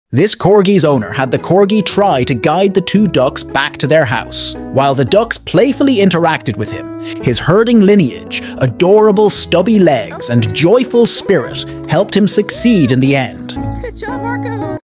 Corgi Herds Two Ducks Back to Their House
This corgi's owner had the corgi try to guide the two ducks back to their house. While the ducks playfully interacted with him, his herding lineage, adorable stubby legs, and joyful spirit helped him succeed in the end.